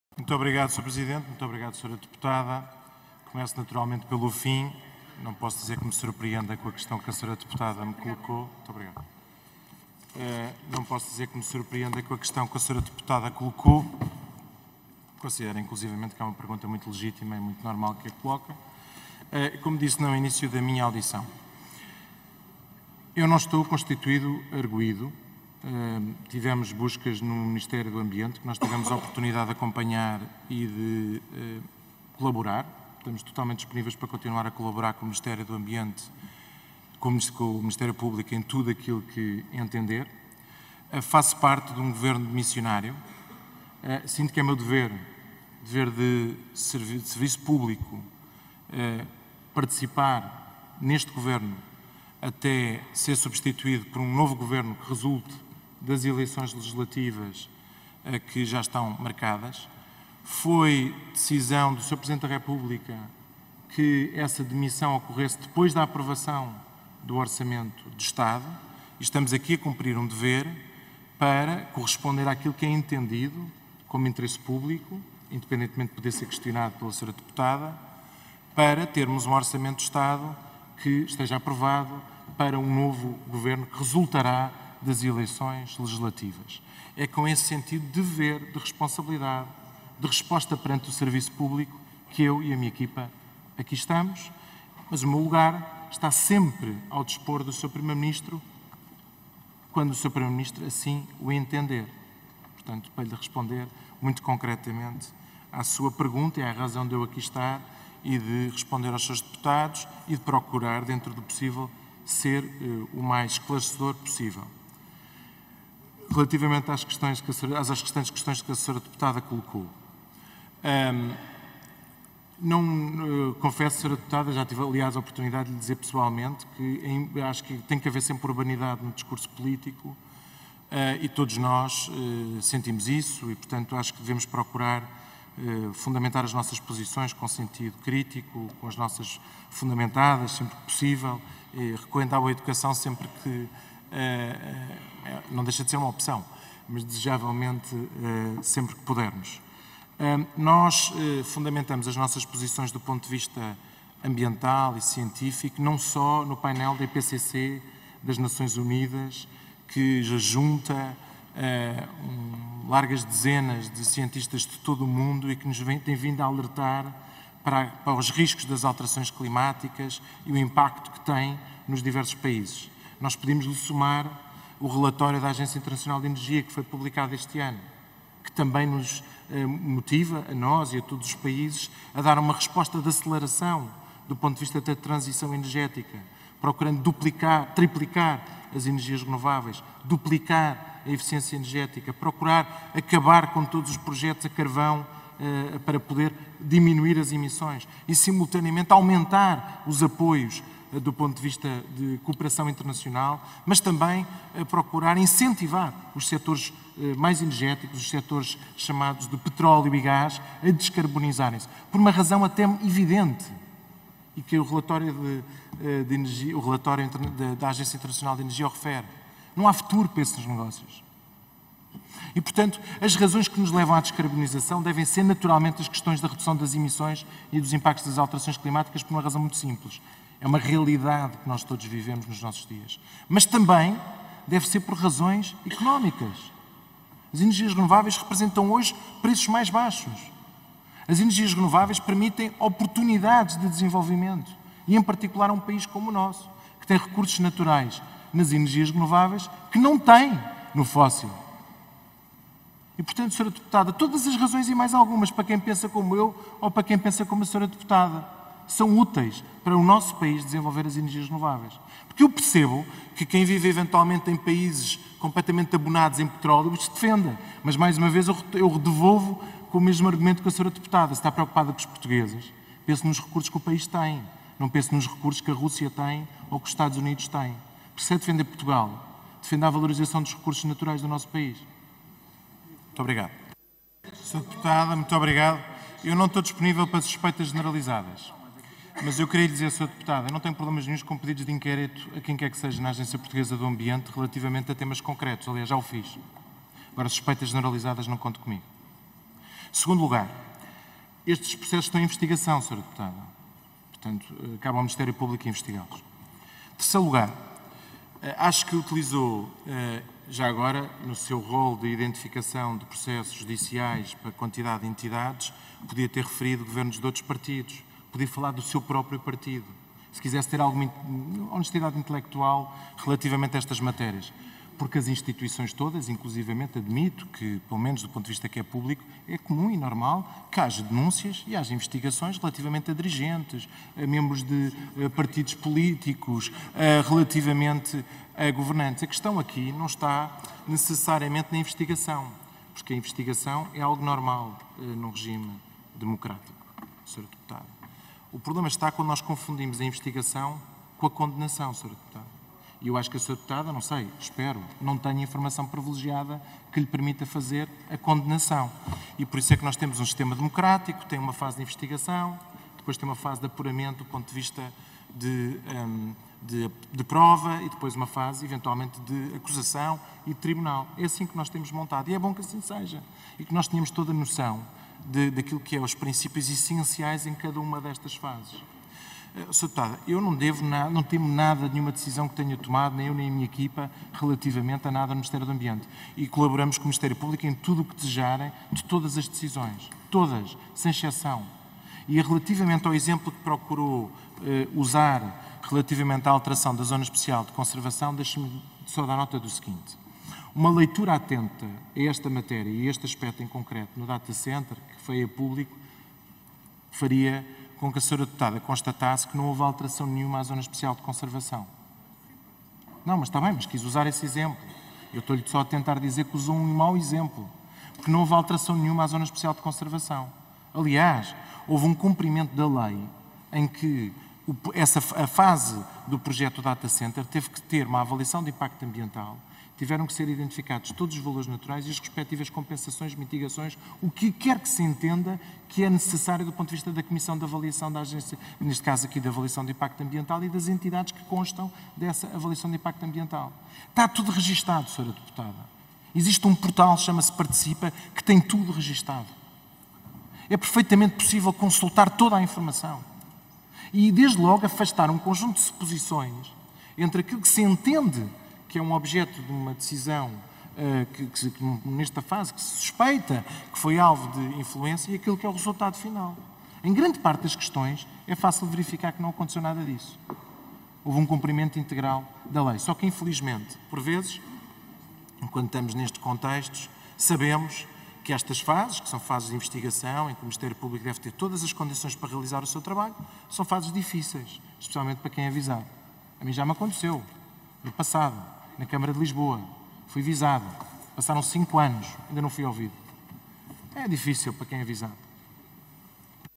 13-11-2023 | OE2024 - Respostas do Ministro do Ambiente à deputada Rita Matias
XV Legislatura
Respostas do Ministro do Ambiente e da Ação Climática, Duarte Cordeiro, à deputada Rita Matias (CH), em audição no âmbito da apreciação, na especialidade, da Proposta de Lei (GOV) - Aprova o Orçamento do Estado para 2024 - Comissão de Orçamento e Finanças - Comissão de Ambiente e Energia - Comissão de Agricultura e Pescas